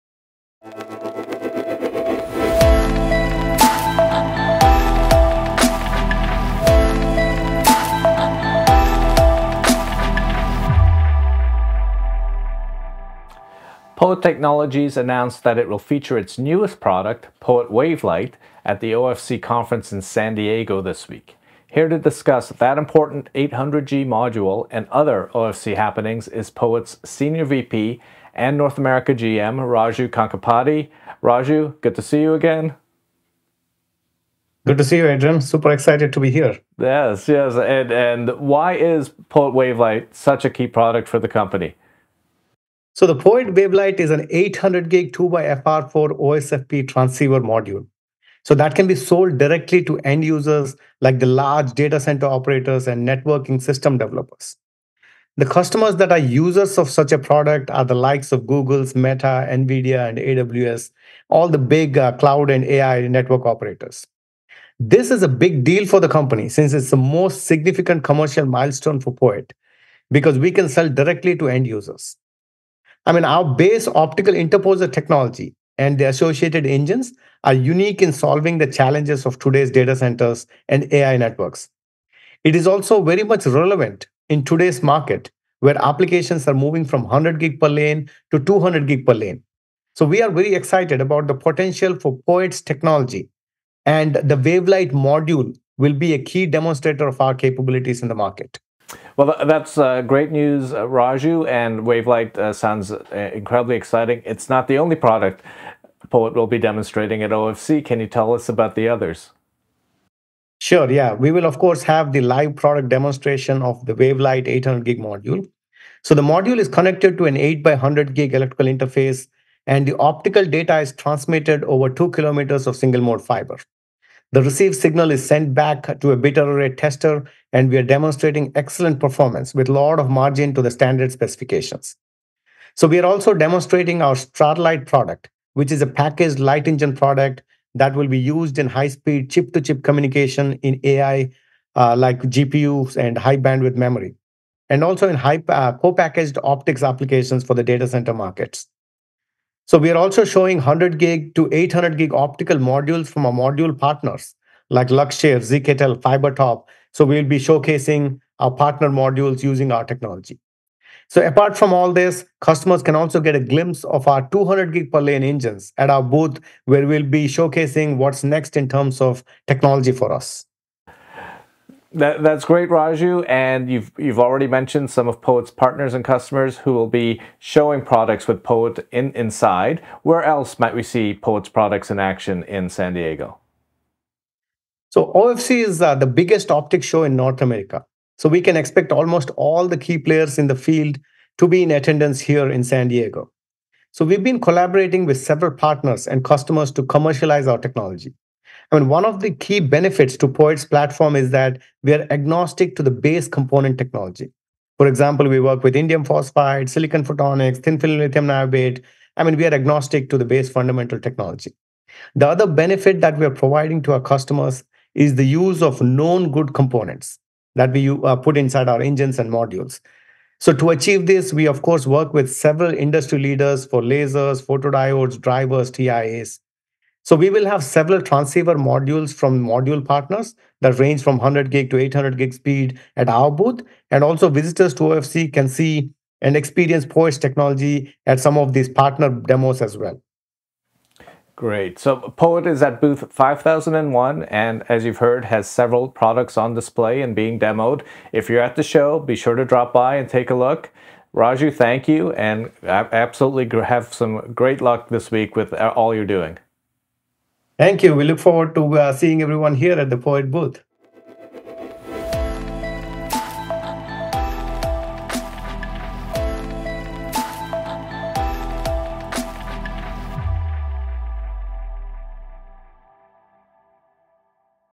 POET Debuts 'The Most Significant Commercial Milestone in the Company's History' at OFC 2024
POET Wavelight marks the AI hardware maker's entry into the module industry, explains Senior VP of Product Management and North America GM Raju Kankipati.